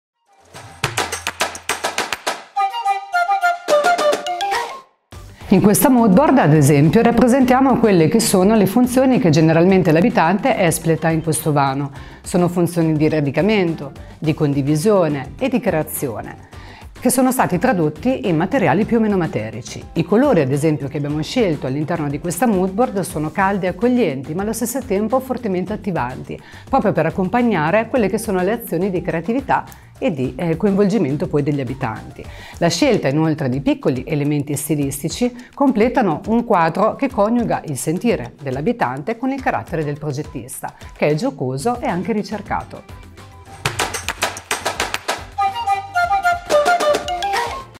Moodboard cucina
Una moodboard per la tua cucina!
Colori caldi e accoglienti che raccontano un ambiente dedicato al nutrimento, ma anche al gioco e alla condivisione.
Realizza il tuo progetto di interni con le nuove moodboard presenti nell’archivio materiali di ArredoCAD.